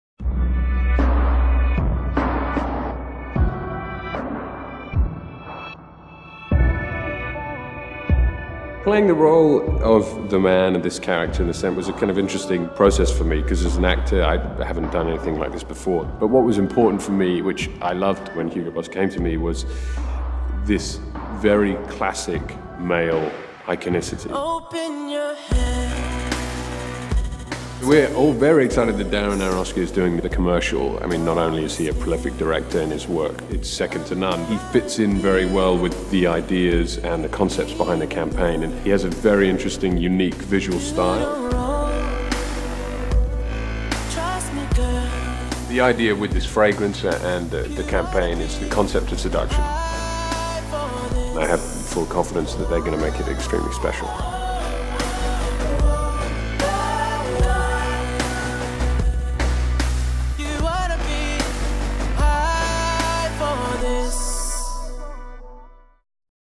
Hugo Boss The Scent - Theo James & Natasha Poly Behind The Scenes - Spanish Sub
Hugo Boss ha presentado su esencia con el actor inglés Theo James,  el cual fue protagonista en la película Divergente. Durante el rodaje del anuncio se observa al actor delante del espejo junto a la modelo rusa Natasha Poly.

The Scent Hugo Boss. “la fragancia más seductora”.